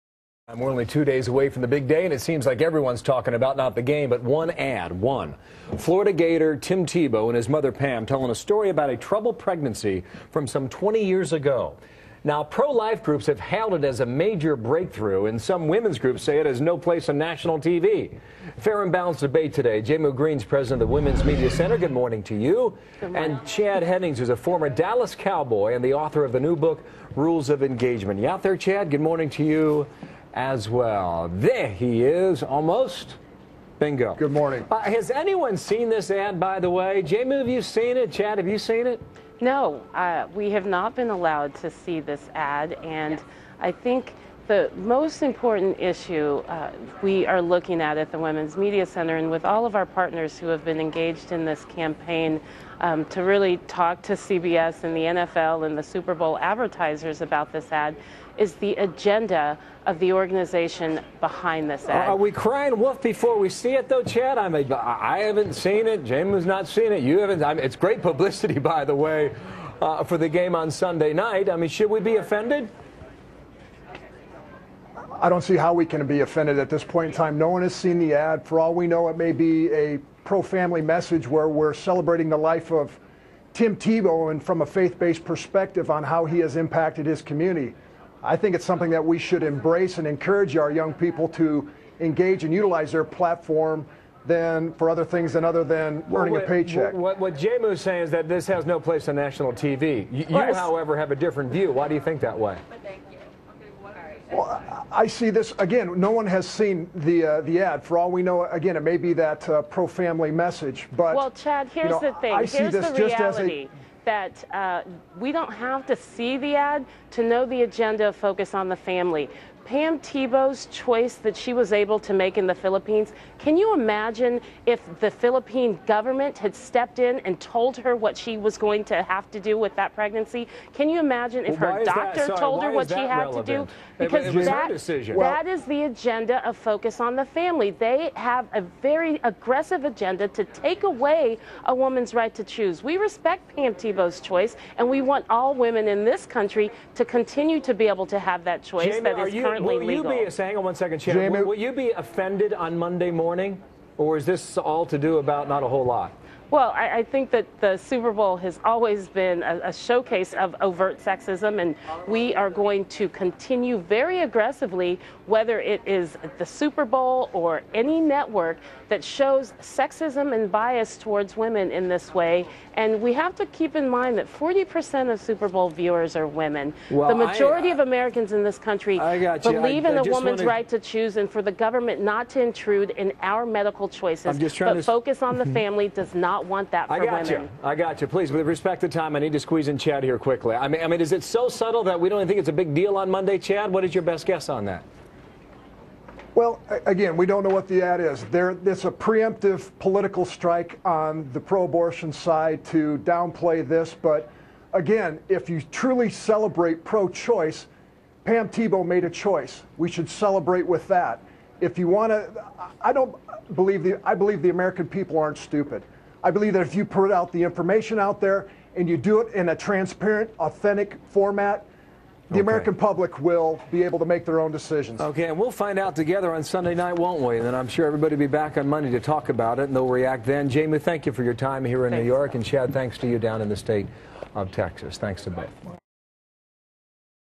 FOX News - Chad Hennings Tim Tebow Super Bowl Commercial
Chad comments on Tebow Super Bowl Commercial